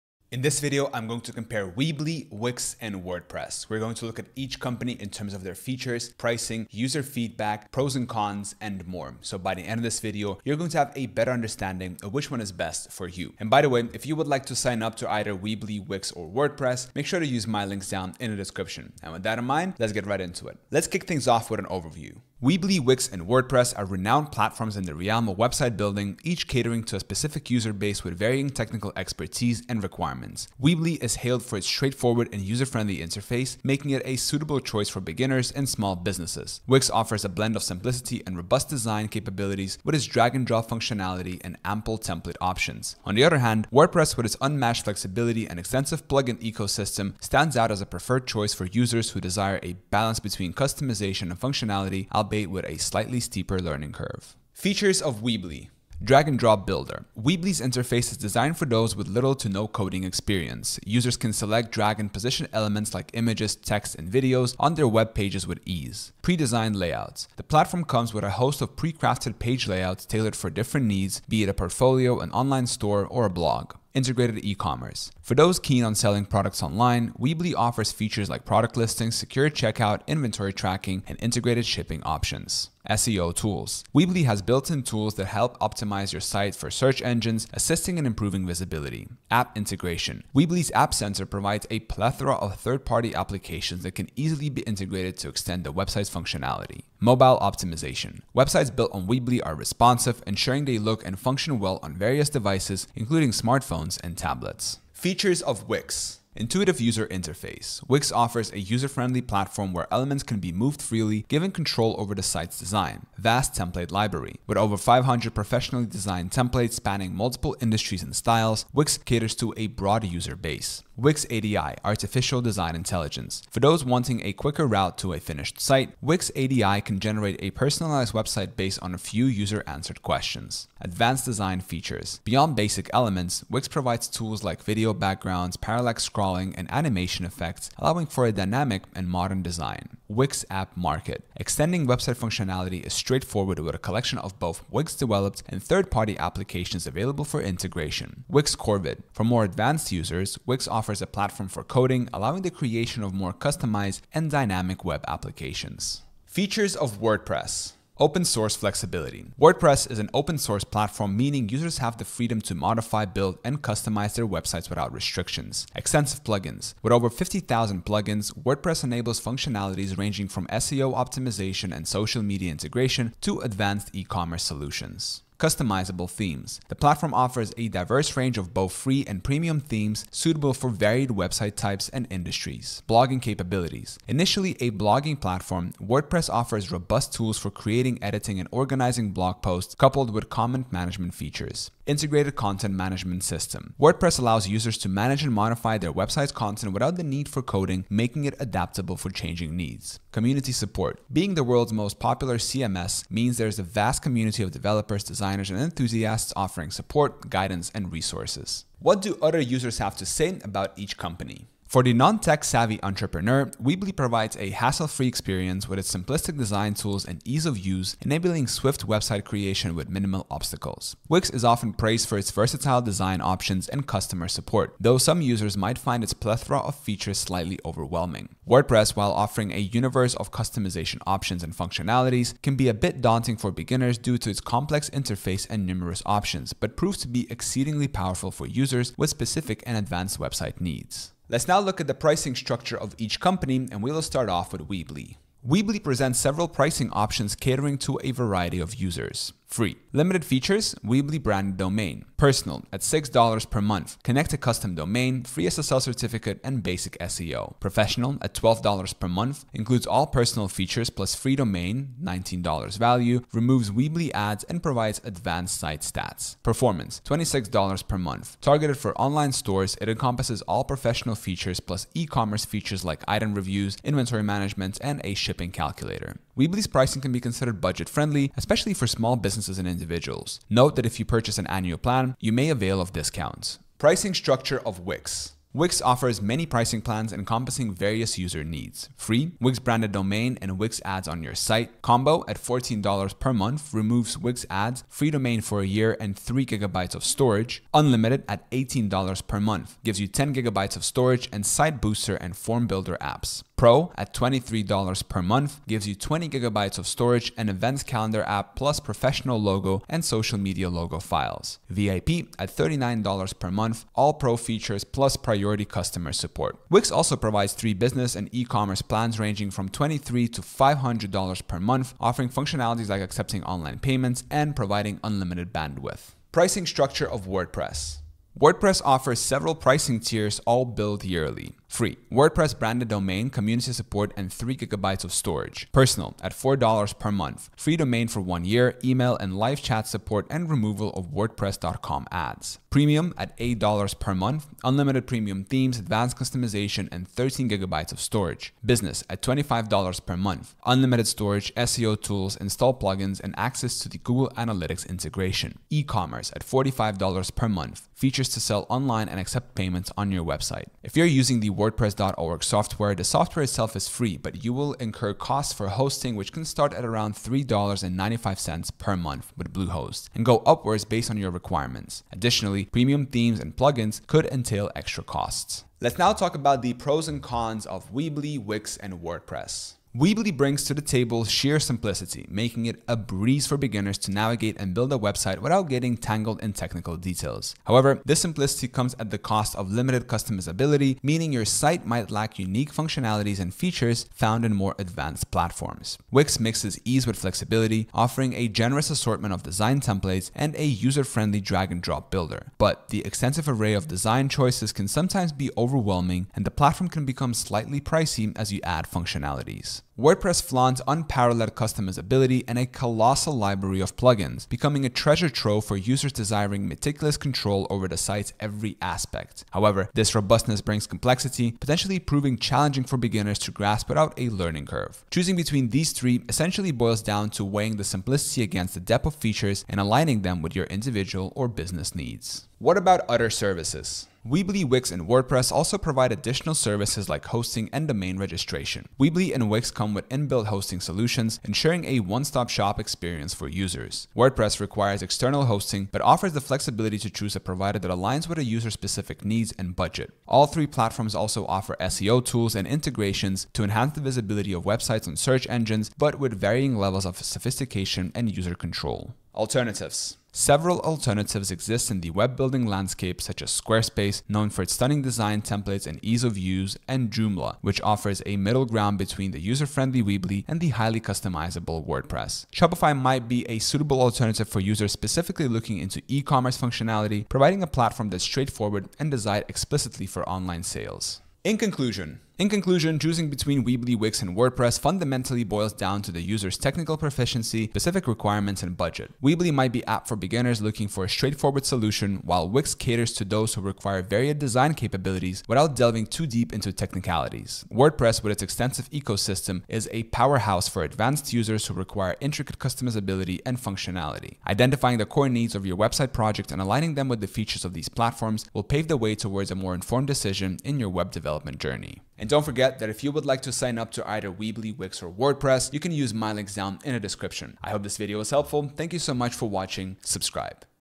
Weebly vs Wix vs Wordpress: Which is Better? (2026)
In this video I compare weebly vs wix vs wordpress.

00:00 Introduction
00:20 Overview
00:54 Feature of Weebly
01:44 Feature of Wix
02:35 Feature  of  WordPress
03:28 User Experiences
04:00 Pricing Structure
06:15 Pros and Cons
07:12 Other Services
07:39 Alternative
08:01 Conclusion